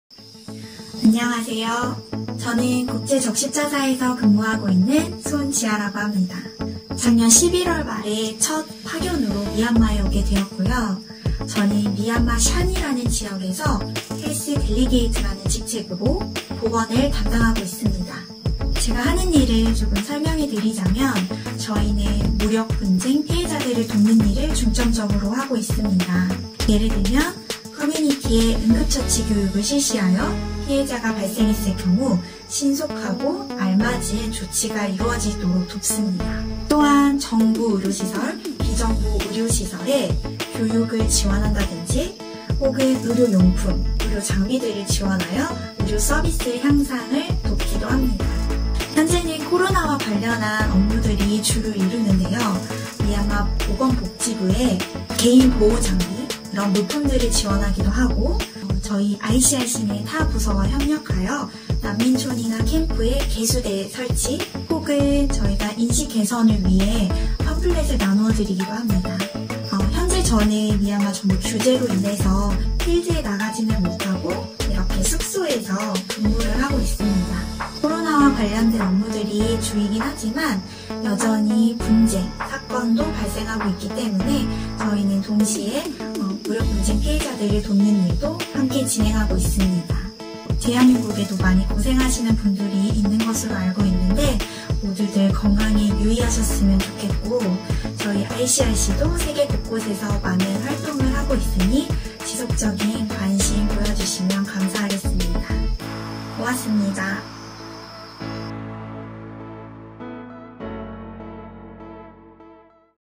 [전세계 ICRC 한국인 직원들] 미얀마대표단의 손지아 보건 전문관
손지아 보건 전문관은 미얀마 샨(Shan) 지역에서 당국과 긴밀히 협엽하여 미얀마의 코로나바이러스 대응 강화 및 보건 서비스 향상을 위해 일하고 있습니다. 장기화된 팬데믹(Pandemic)으로 현장에 대한 접근이 어려워진 상황에서도 손 전문관은 지역사회가 전염병을 예방할 수 있도록 위생시설을 설치하고, 지역 사람들의 인식제고를 위한 다양한 활동을 지원하고 있습니다.